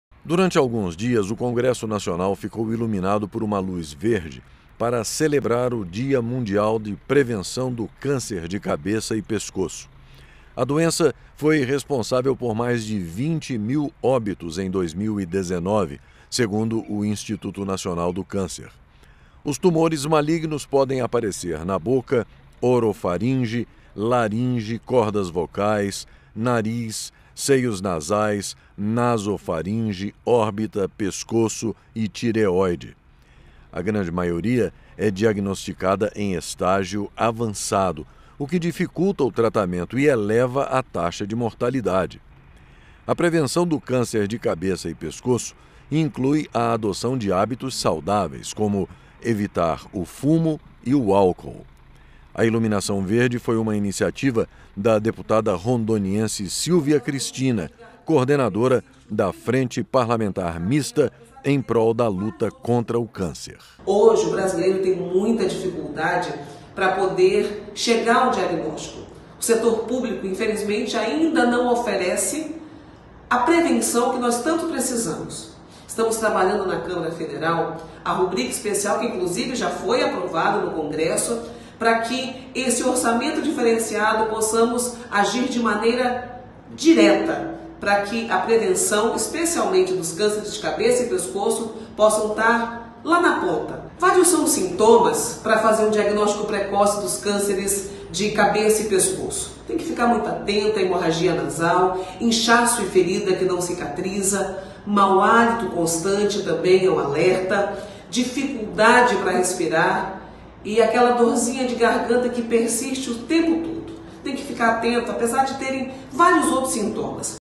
Congresso fica verde pela prevenção do câncer - 05/08/22
O Legislativo chamou atenção para a necessidade de prevenção dos casos de câncer de cabeça e pescoço. A campanha iluminou de verde o prédio do Congresso Nacional.